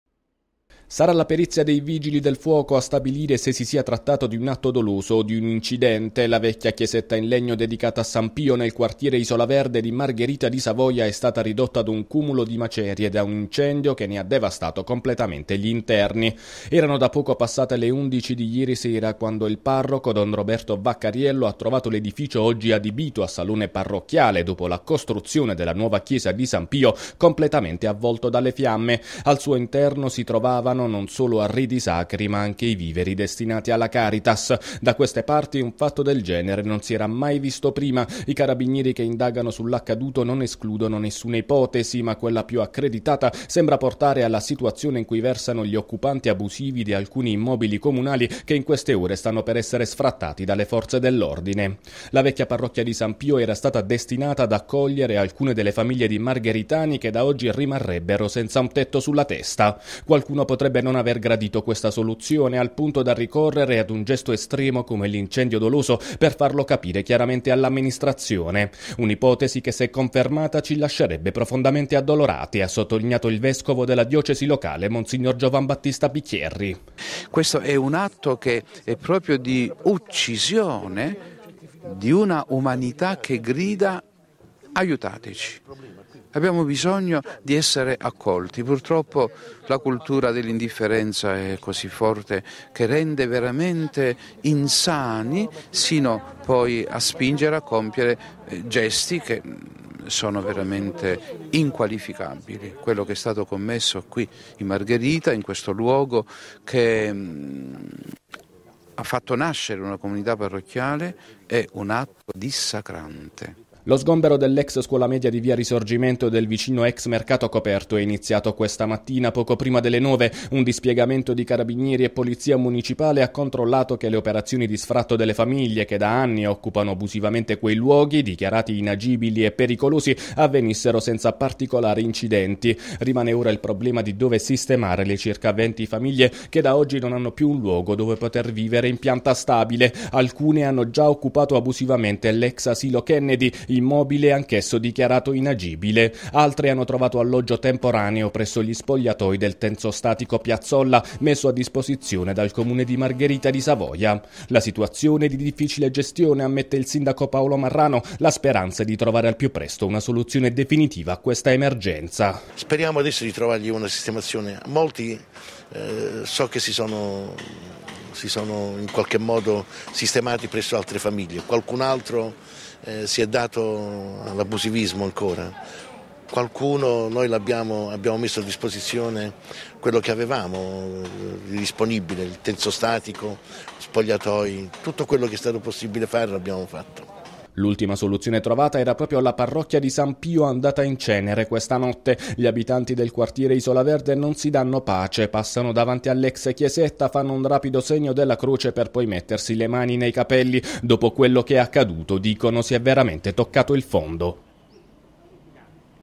Margherita | L'ex chiesa di San Pio distrutta da un incendio: era destinata agli sfrattati
L'ex chiesa di San Pio, nel quartiere Isola Verde di Margherita di Savoia, è stata distrutta ieri notte da un incendio probabilmente doloso. La struttura era destinata ad accogliere alcune famiglie che occupavano abusivamente gli immobili comunali.